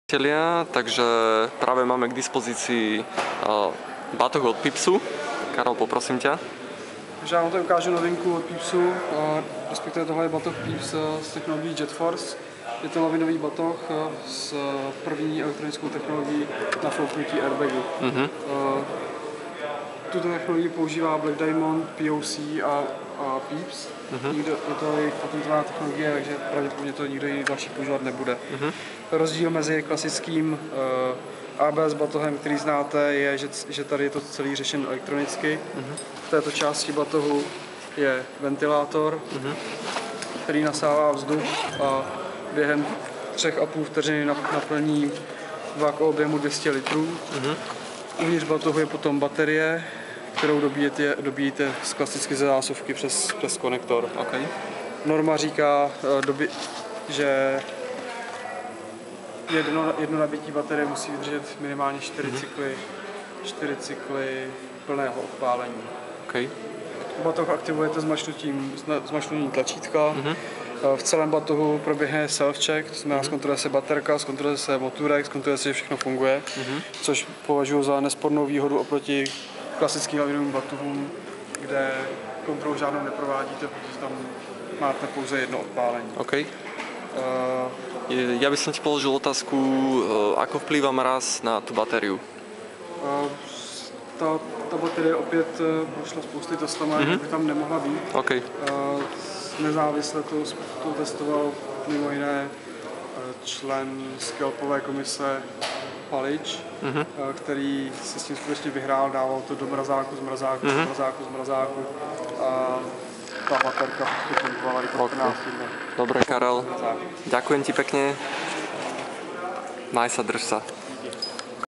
Lavinový batoh Pieps Jetforce | Predstavenie
Krátka prednáška o tomto lavínovom batohu Jetforce od firmy Pieps, zachytená na outdoor výstave v Čechách. Jedinečnosť batohu spočíva v tom, že ho nenafukuje "plyn" ale elektrický motorček. Má možnost, čekovania nabitia betérii pred "akciou", ako aj niekoľko násobného nafúknutia ruksaku(asi 3-4x) na jedno nabitie batérii.